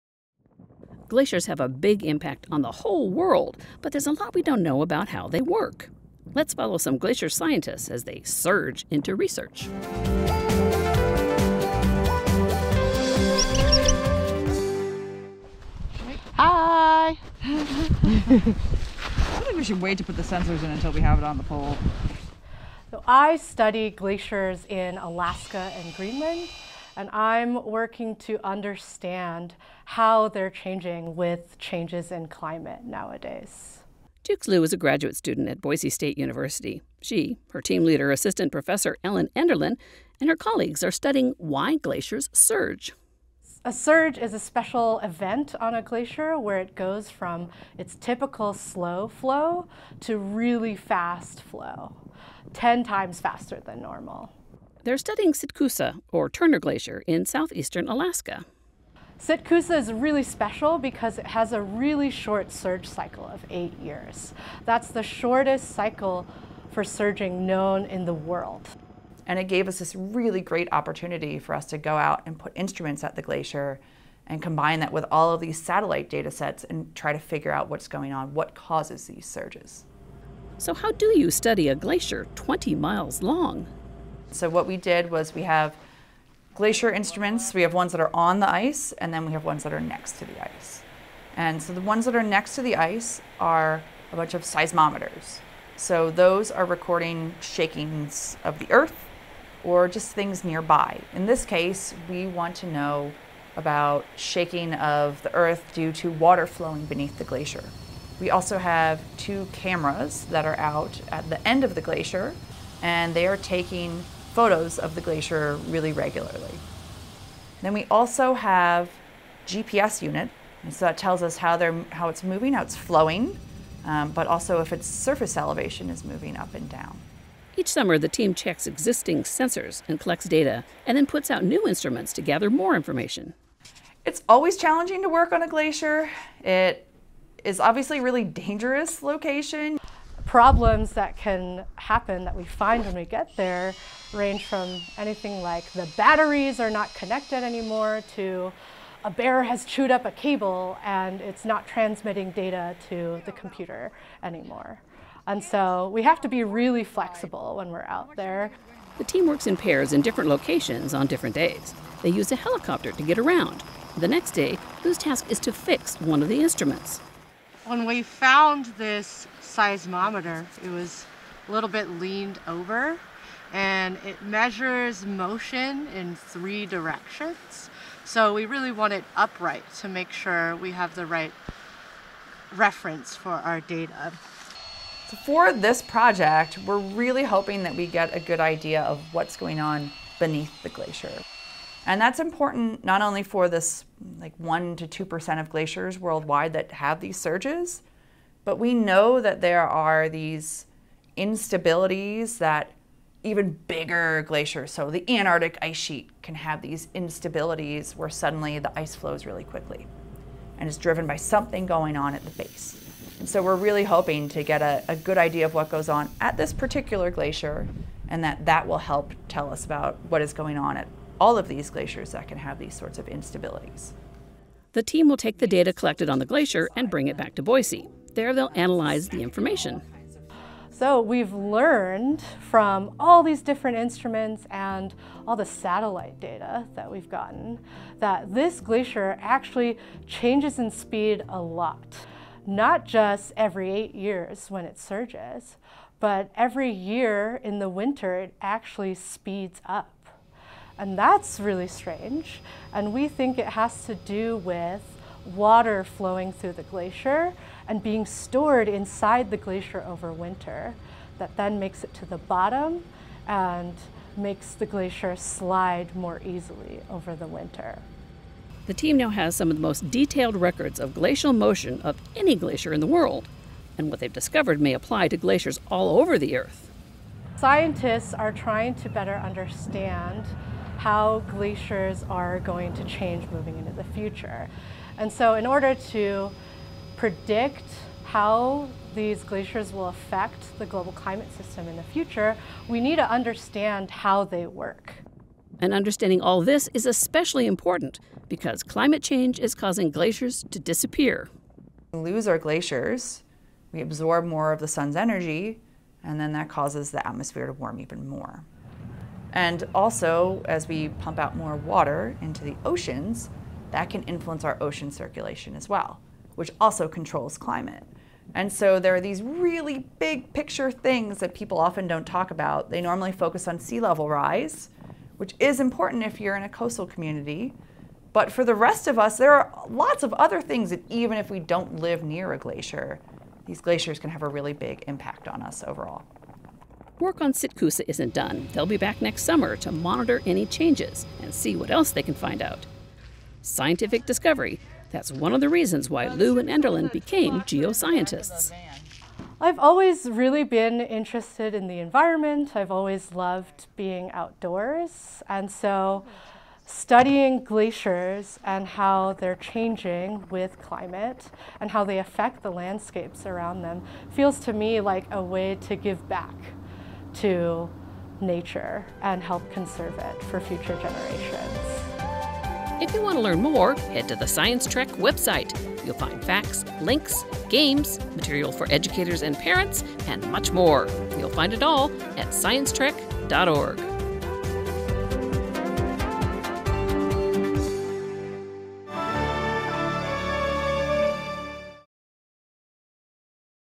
Glaciers: Surges and Melting Glaciers | Science Trek
Glaciers usually move a few inches a year, but scientists at Boise State University are studying one glacier that is moving much faster.  Investigating these surges could help us understand instability in glaciers around the world.  That information could save lives.

Glacier [\'glā–shər]
Large body of slow moving ice

Glaciers are formed from snow and water that gets compressed together over long periods of time. They are made of up ice, snow, rocks, sediment and liquid ice. They move down a slope because of gravity and its own weight. Ancient glaciers carved out much of the Earth's landscape. Glaciers that remain today are lefts over from the Ice Age. Scientists study glaciers to see into the past and understand what may happen in the future. But many glaciers are melting because of climate change. That may one factor in the rise of global sea levels. The health of glaciers can tell us a lot about the health of our planet.

You'll find lesson plans, facts, games, and more on our Science Trek